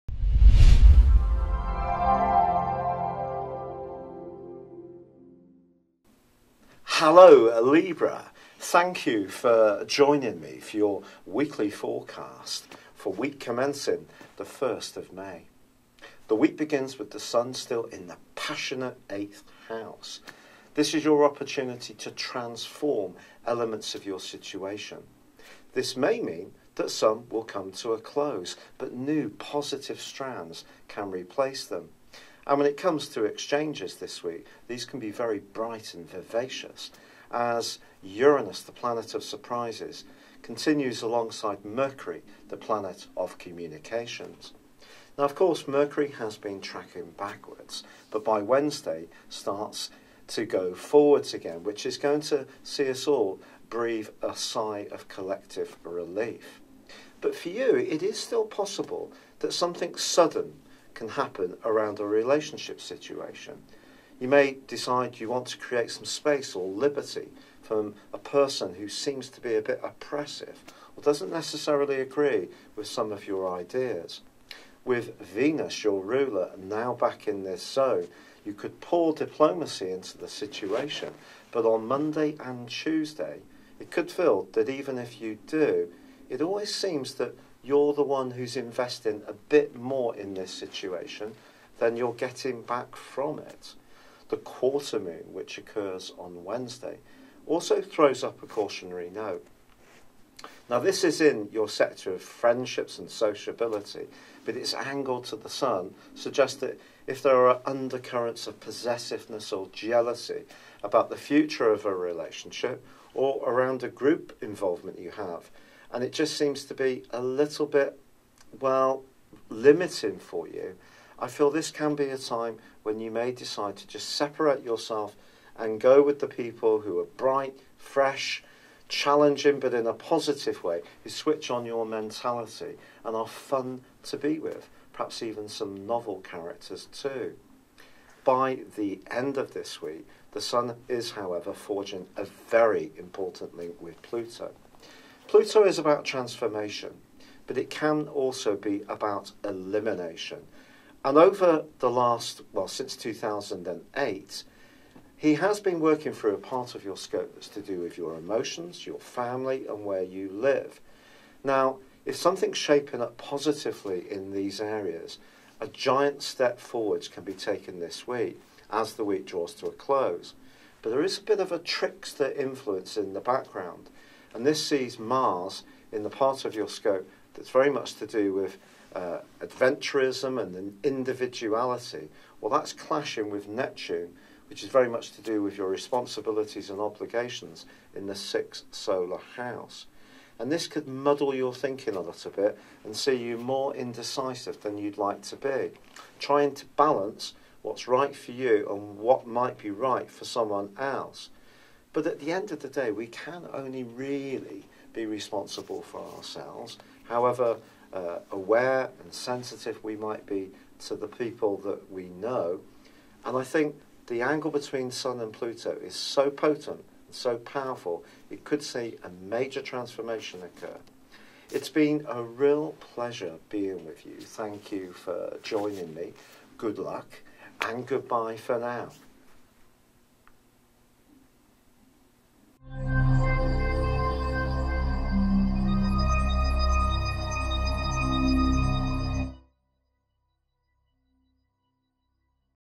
Libra Weekly Horoscope from 1st May 2017
WC 1ST MAY, LIBRA 24 SEPTEMBER - 23 OCTOBER... With your ruler now back in your sector of relating, and Mercury the planet of communication now in forward motion, this week presents an opportunity to get greater clarity about your hopes around key involvements. What's essential is that the balance between what you want and are prepared to give, and those things that you need to nurture you, are not too far out of kilter.